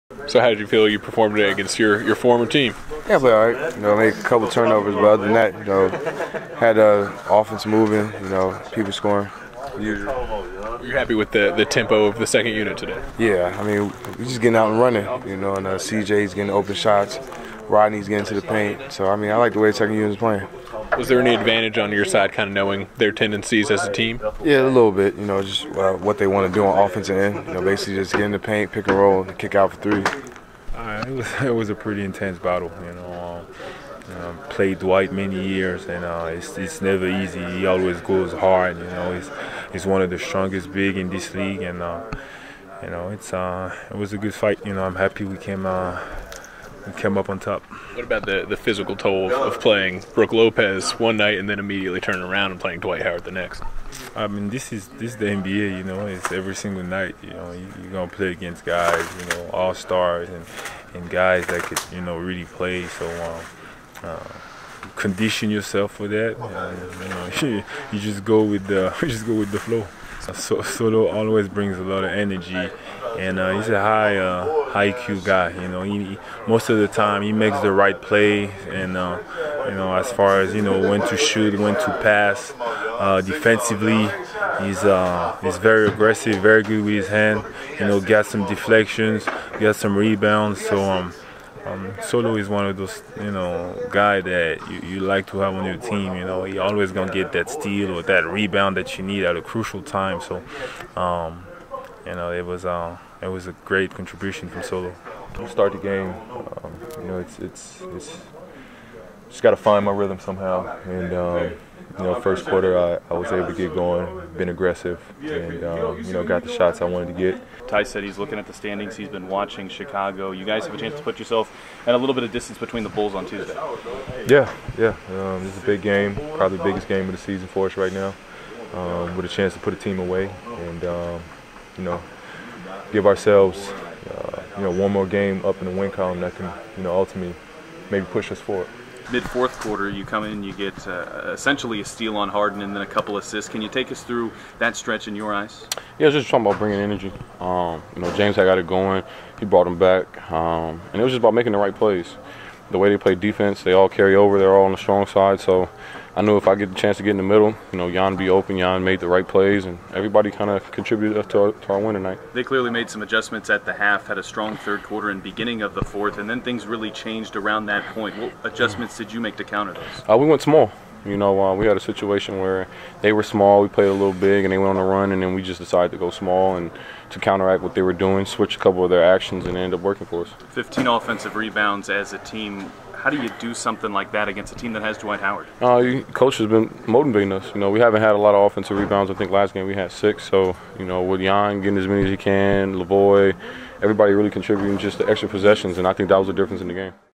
Pacers React to Win Over Rockets
March 27, 2016 - Pacers players Ty Lawson, Ian Mahinmi, Paul George, and Solomon Hill reflect on their 104-101 win over the Houston Rockets at Bankers Life Fieldhouse on Sunday evening.